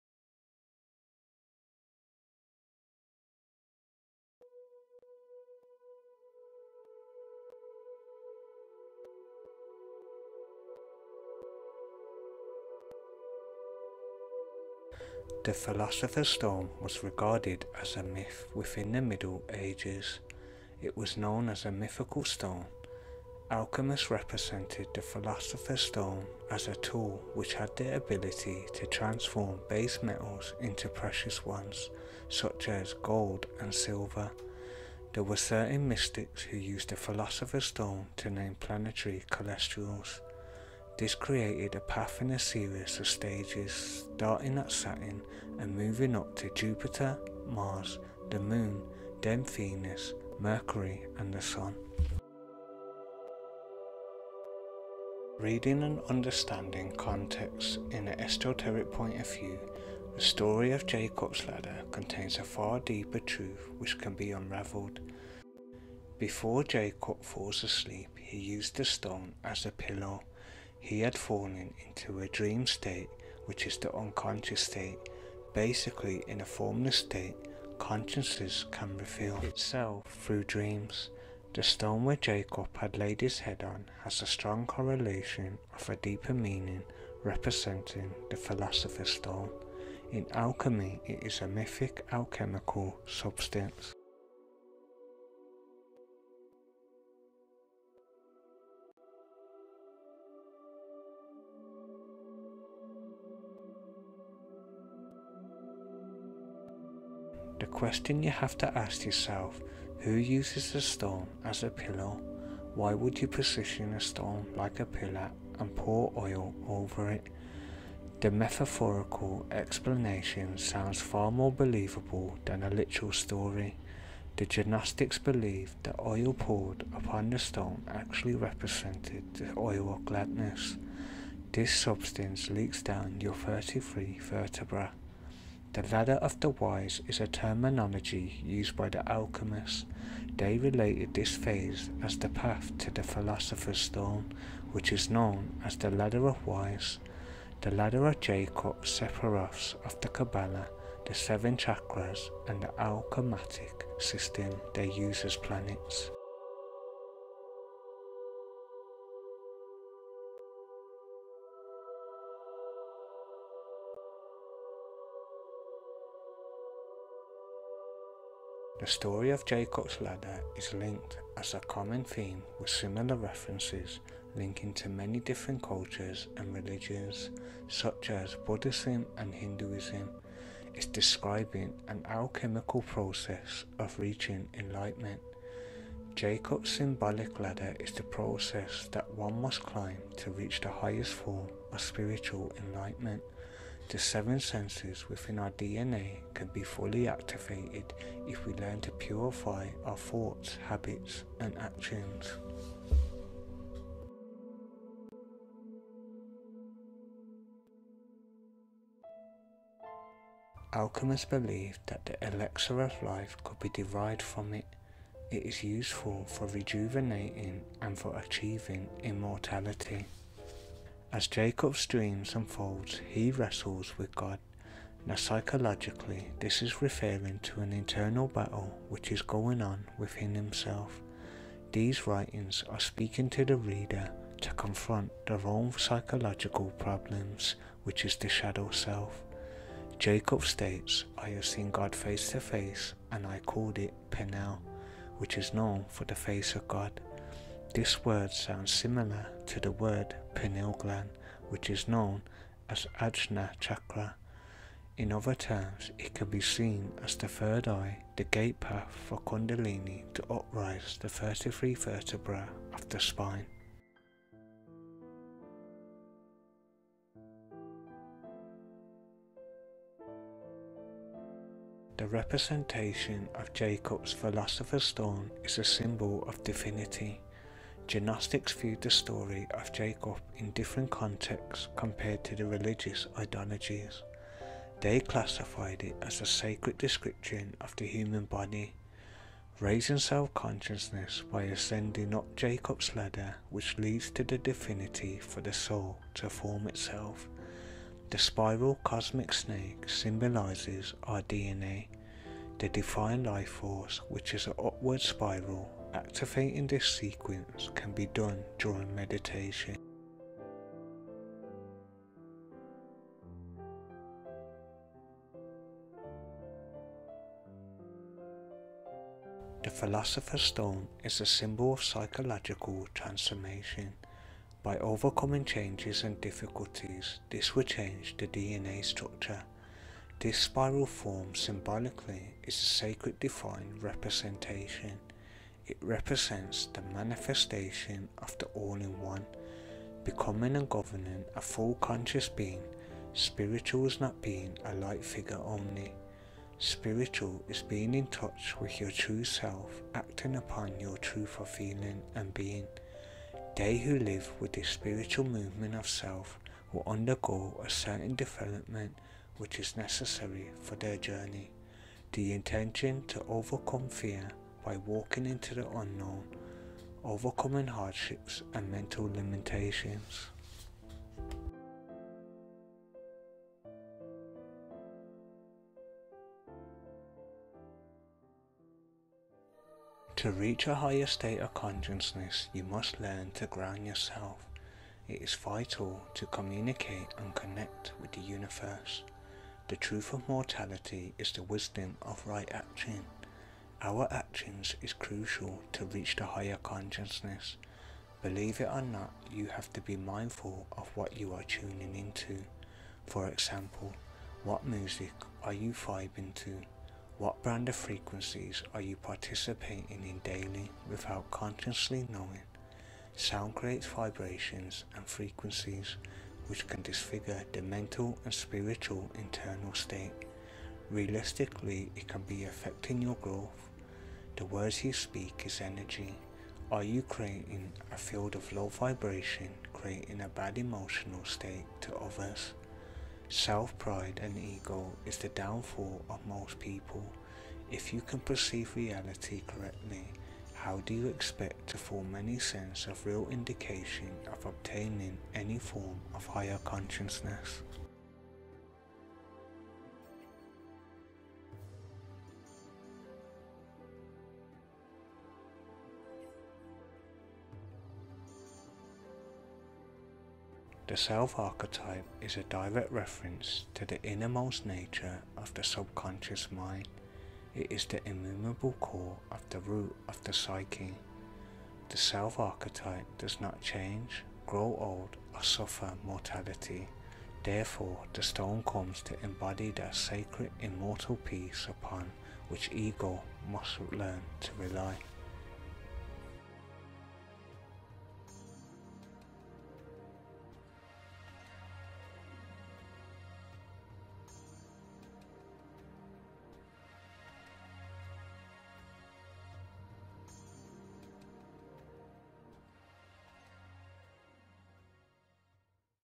The philosophers stone,Ascending Jacobs ladder[metaphorical,alchemist of the human body]
Reading bibical scriptures through the eyes like a mystic you will discover profound proof,that Jacobs ladder has a deeper meaning,by using symbolism and alchemy the story of jacobs ladder is more metaphorical than literal,as its describing a humans physiological mental state of being,this video will dive deep into occult knowledge breaking down the fundamental aspects explain esoteric,kundalini knowledge of the gnostics.